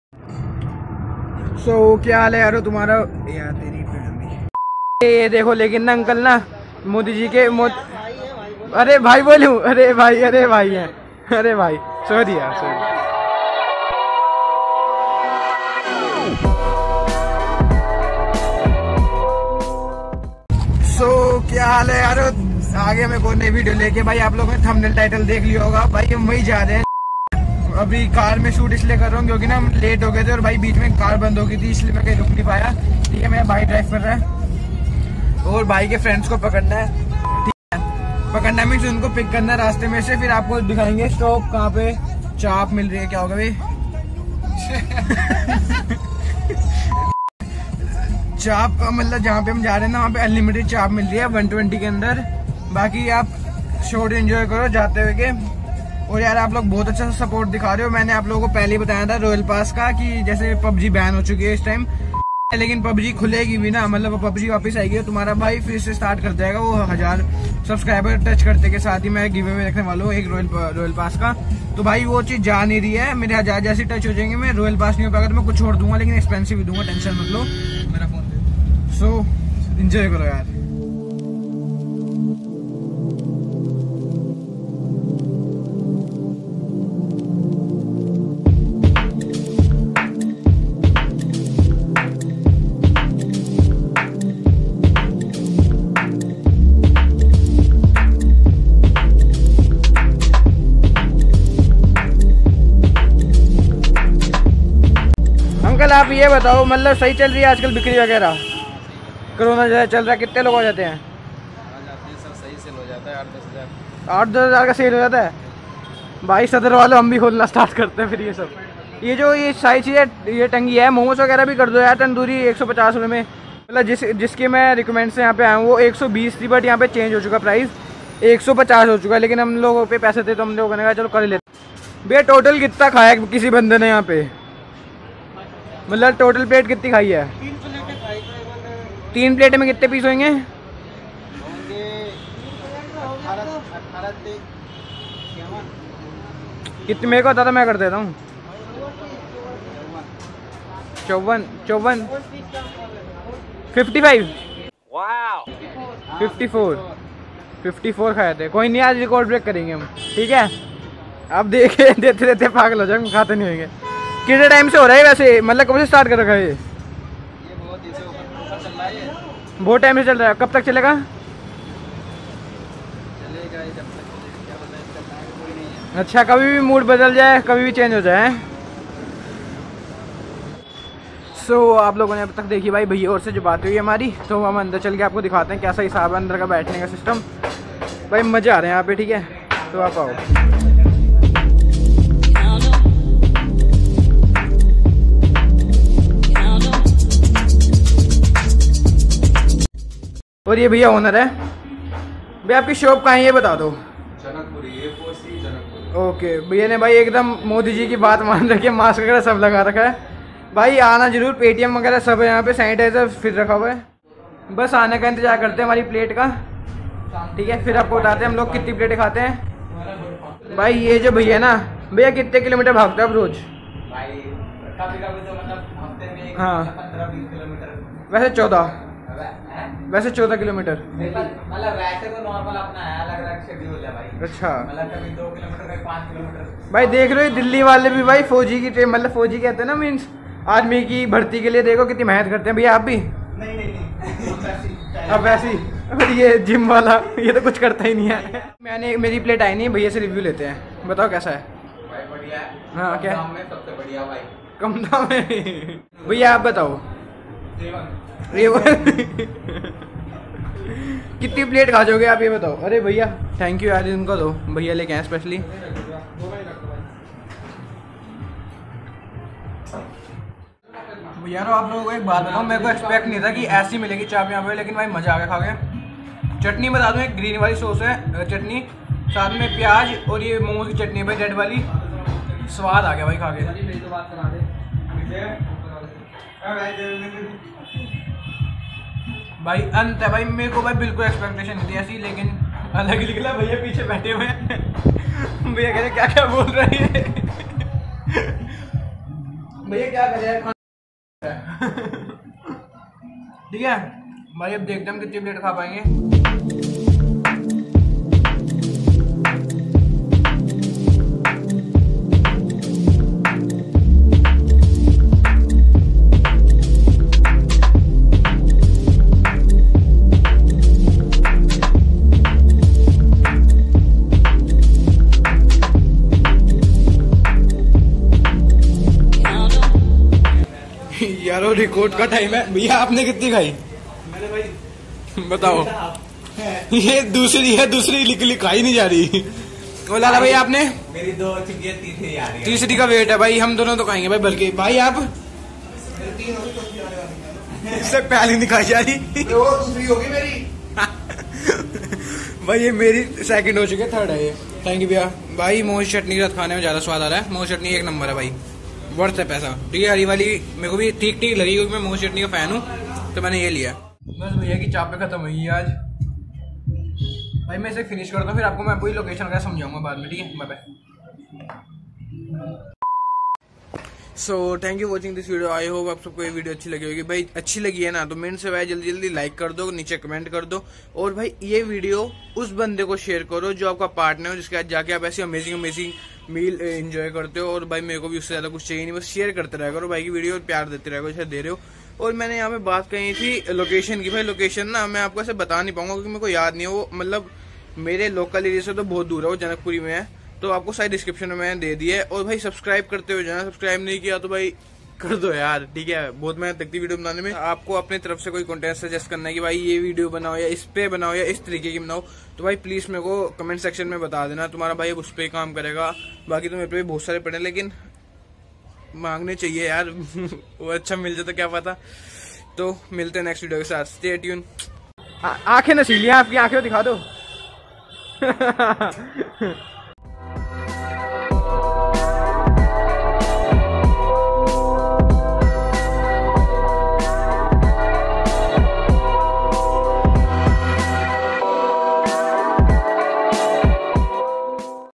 UNLIMITED CHAAP 😲 | Spicy Hut Chaap Janak Puri | Delhi Food Vlog - Anirudh Malhotra Vlogs
--Welcome To Anirudh Malhotra Vlogs--

Having Unlimited Chaap with Friends

Shop Name - Spicy Hut
Address - A-4C/193A, Janakpuri, New Delhi - 110058

Opening Hours : 3:00pm to 10:00pm

Price : UNLIMITED Chaska Chaap - 150 Rs